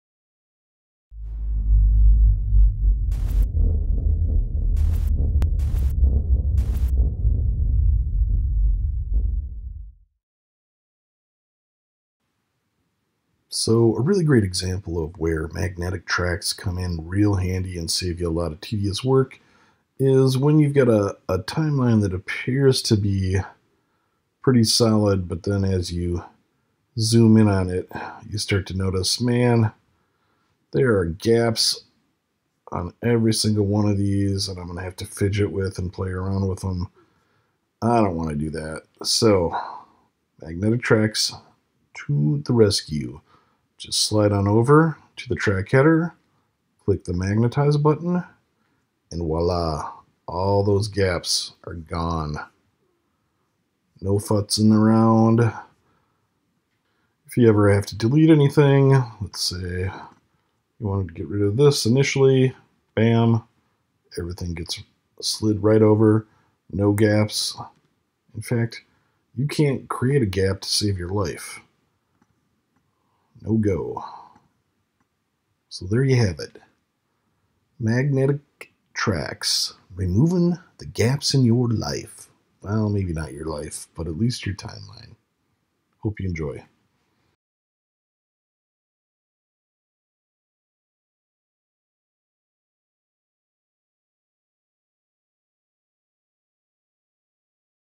Camtasia 2020 - Magnetic Tracks: Quickly Remove Gaps Between Media
Removing gaps between media clips on a track can be tedious and time consuming. Magnetic Tracks can turn that frown upside down. Learn how simple it is to leverage this new capability in Camtasia 2020. Your time and your wrists will thank you.